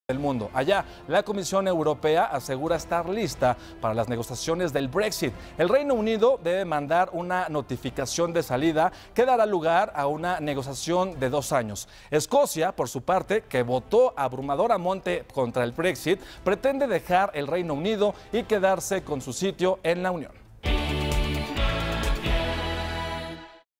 Viene el 'Brexit' y Escocia quiere zafarse
Vuelven a pedir su independencia del Reino Unido.